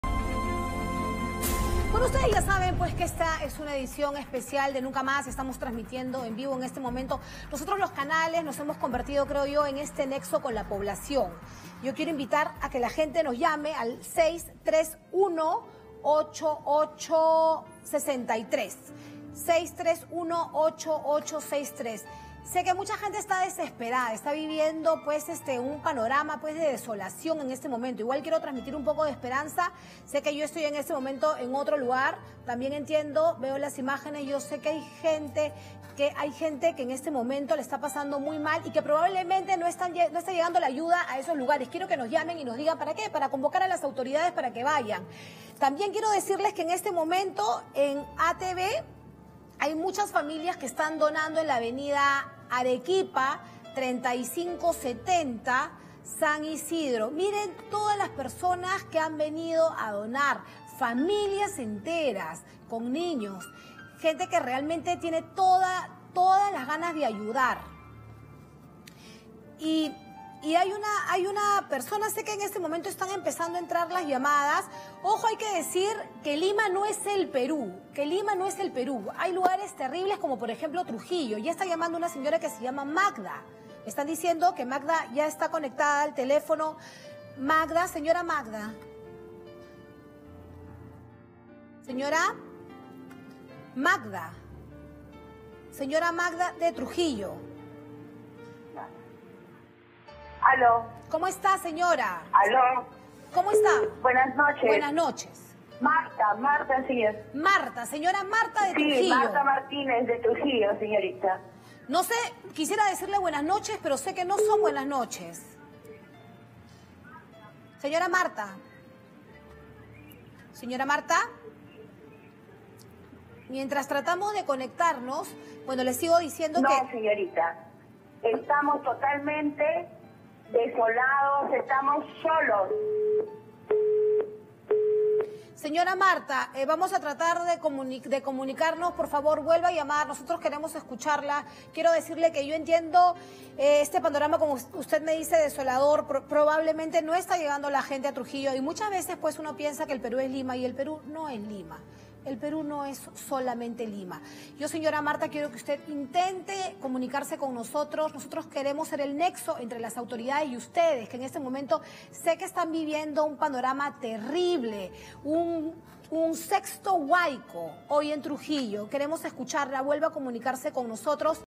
Grupo ATV realiza llamado a seguir apoyando a damnificados
La emergencia que afecta a nuestro país, ha sido abordada por todos los canales de televisión, si bien el deber es informar también hemos querido aportar nuestro granito de arena, pensando en todas las familias que han perdido completamente todo. Por este motivo es que habilitamos una carpa hace varios días, ubicada en las instalaciones de nuestro canal en San Isidro, dirección Av. Arequipa 3570. En la que puedes traer tus donaciones de alimentos no perecibles, agua embotellada, pañales, frazadas y ropa para todas las edades.